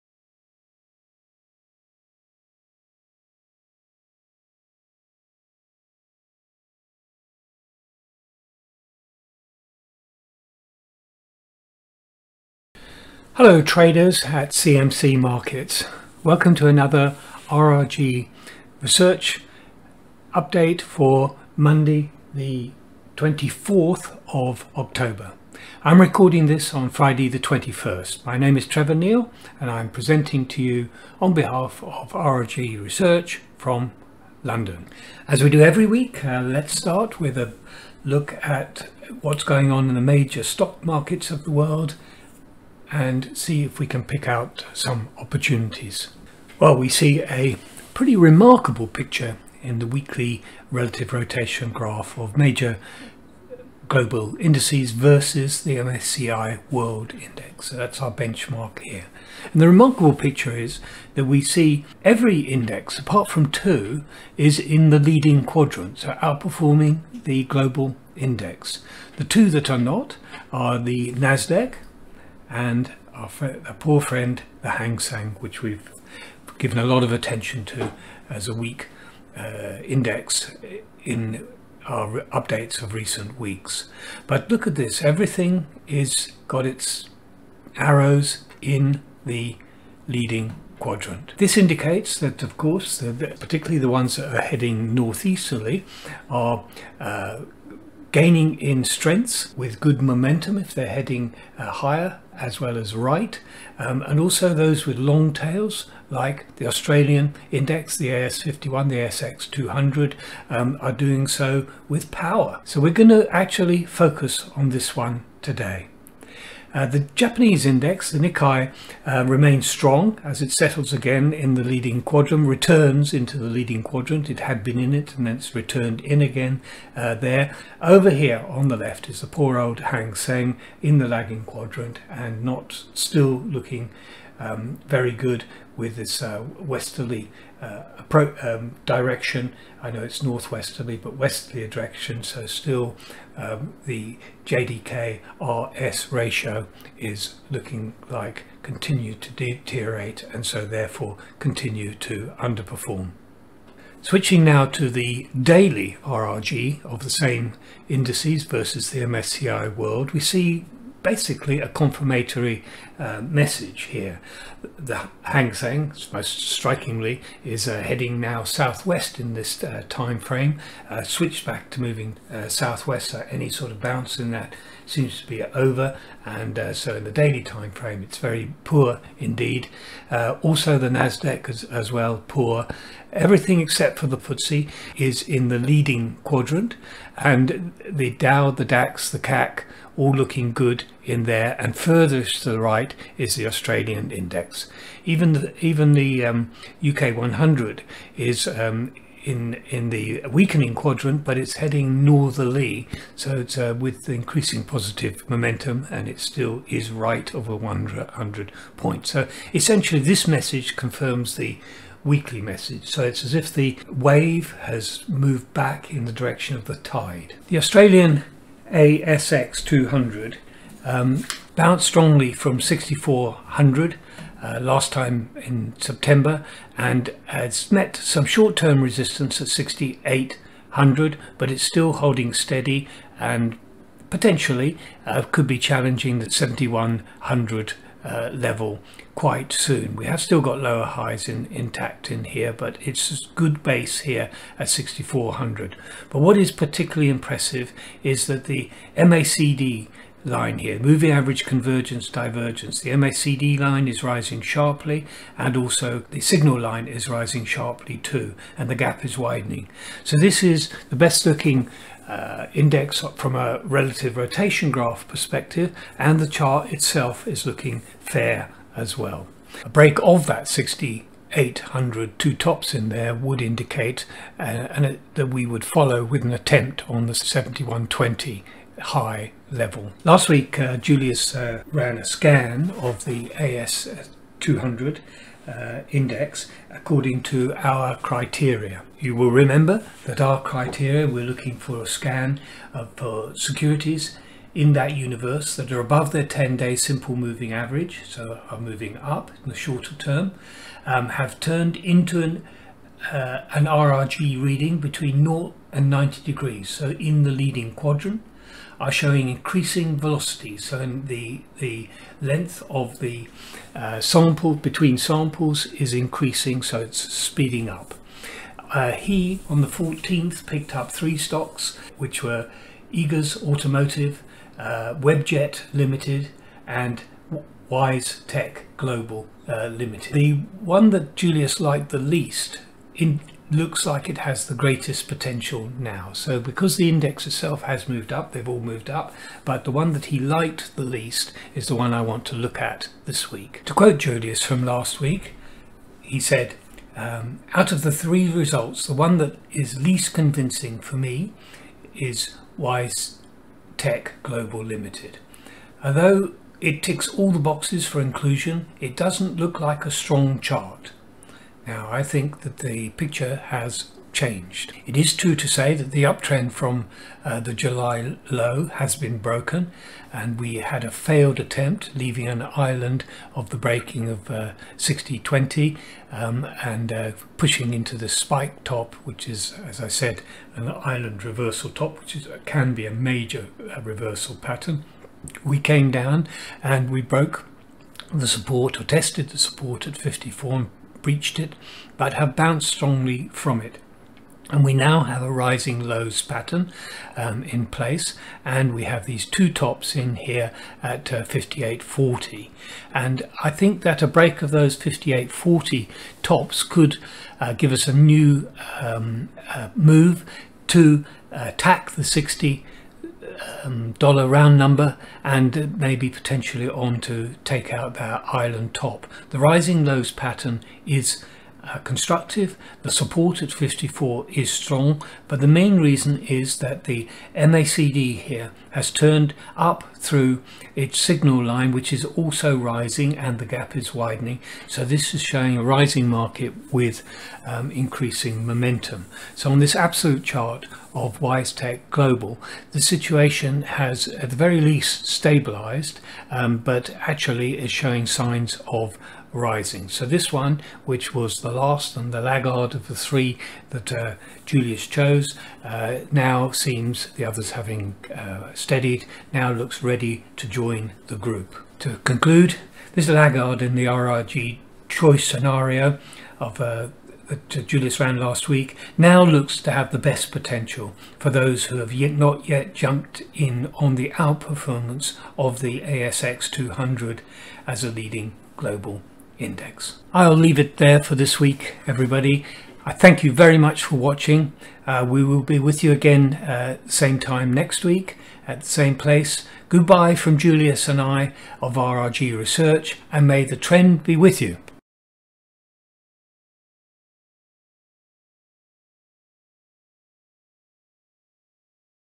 Stock to watch this week: Wisetech Global Limited | RRG X CMC Markets
This week, Trevor Neil revisit’s Australia’s ASX index and interestingly sheds light on how last week’s laggard stock, Wisetech Global (WTC AU Equity) now looks to have the best potential for those who have not yet jumped on the outperformance of the ASX200 as a leading global index.
Indeed, the uptrend from the July low has been broken. The action now looks like an ascending triangle – usually a bullish formation. A break of the two tops at 58.50 will confirm it.